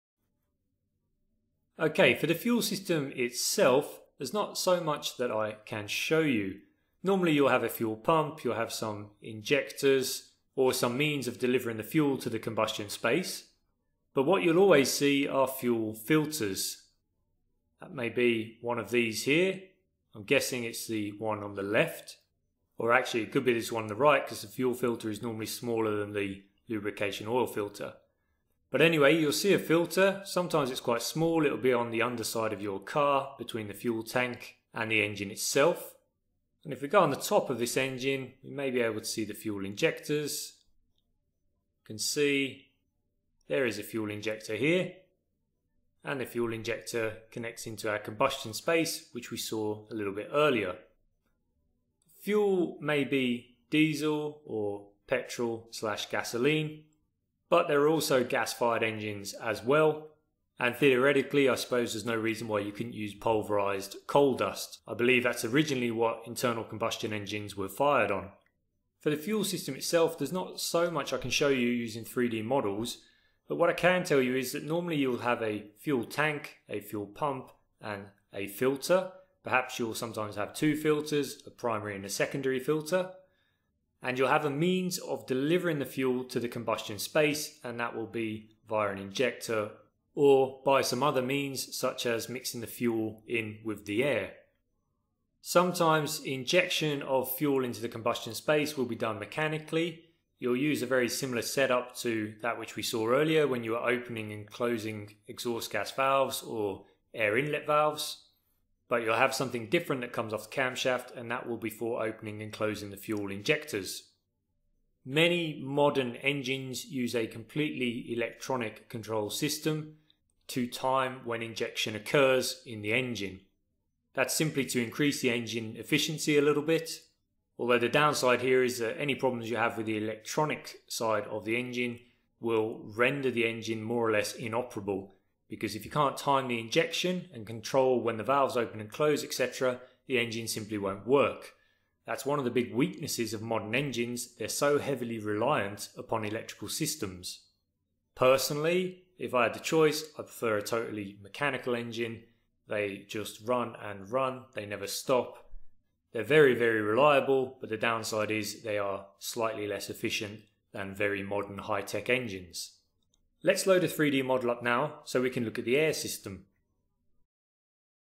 (3D) IC Engine Fuel System / How does it work?
Welcome to our informative video on the (3D) IC Engine Fuel System and how it works! In this video, we will delve into the intricacies of internal combustion engine fuel systems, providing a comprehensive understanding of their functioning.

🔧 Learn the Basics of IC Engine Fuel Systems: From Fuel Injection to Combustion 🔧

The (3D) IC Engine Fuel System is a critical component of modern combustion engines, responsible for delivering the right amount of fuel to the engine cylinders for efficient combustion. Understanding how this system operates is essential for any automotive enthusiast, mechanic, or curious learner.

💡 Discover the Advantages of (3D) Visualization 💡

By utilizing cutting-edge (3D) visualization technology, we bring you a unique learning experience. Our high-quality visuals allow you to examine the fuel system from multiple angles, making it easier to grasp complex concepts. Whether you're a student, a professional, or simply an automotive enthusiast, this video will provide you with valuable insights into the inner workings of IC Engine Fuel Systems.